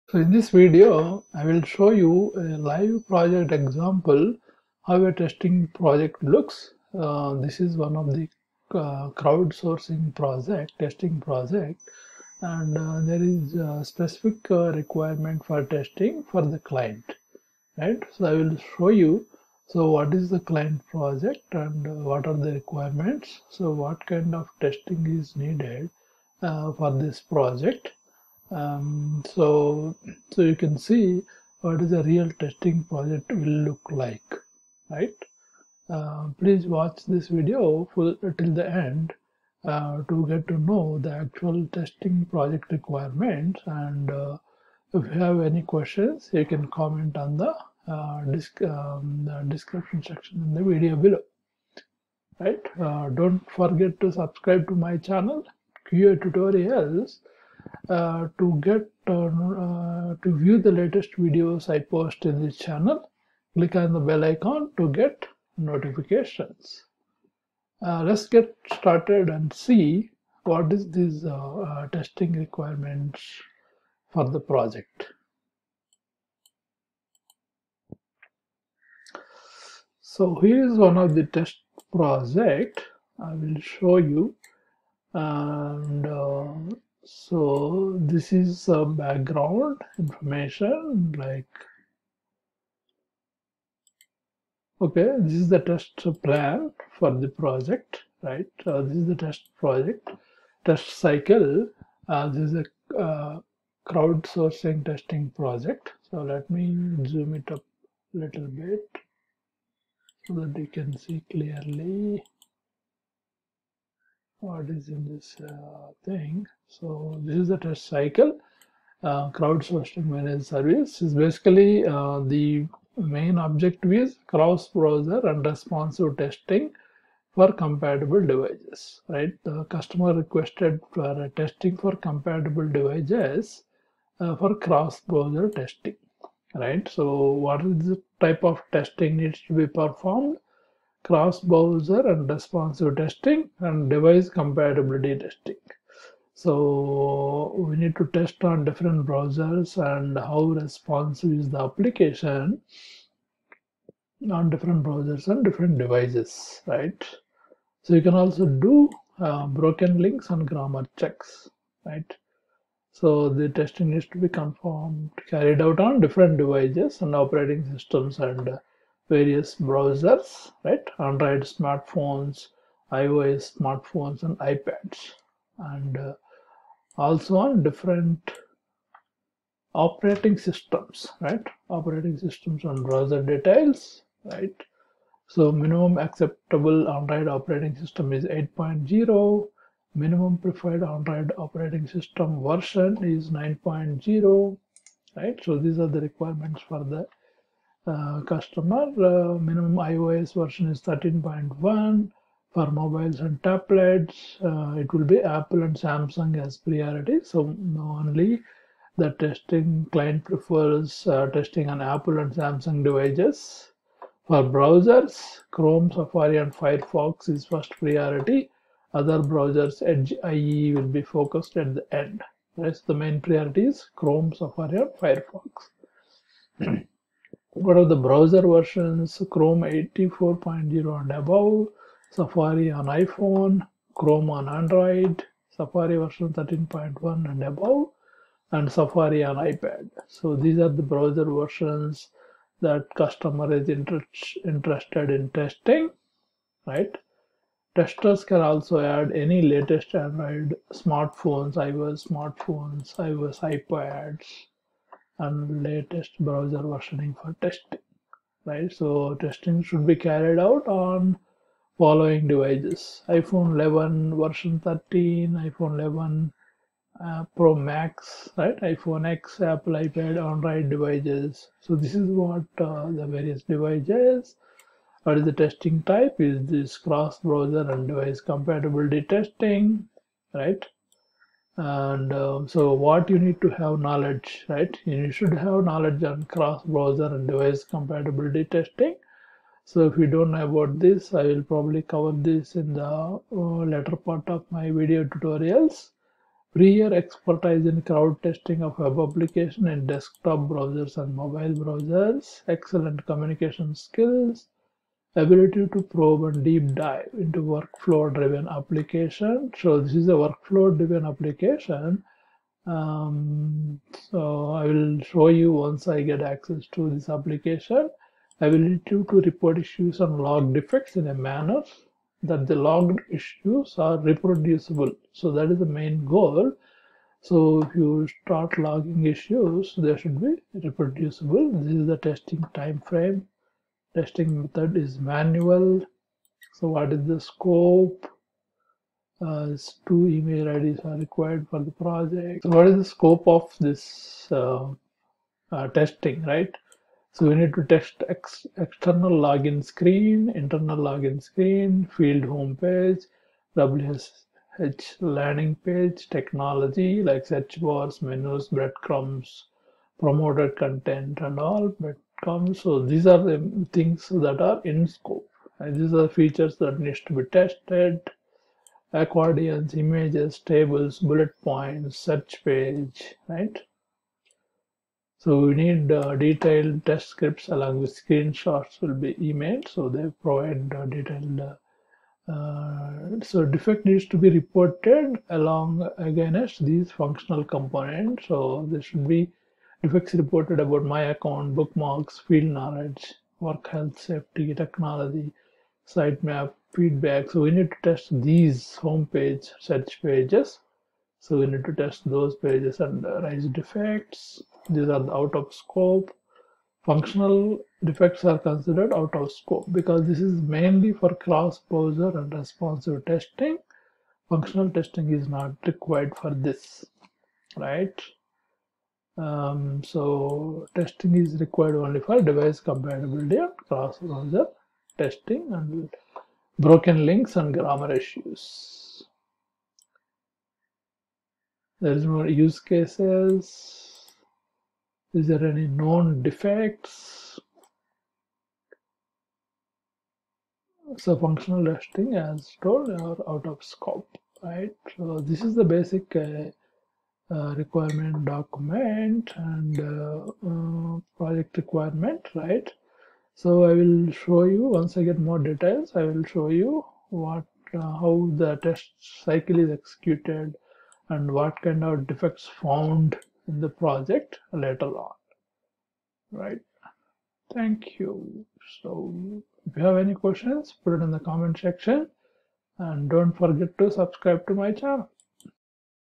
Manual Testing Live Project
In this video,  I will show you Manual Testing Live Project example and what are testing requirements for the project.

Follow me on different platforms to stay updated.

Category
People & Blogs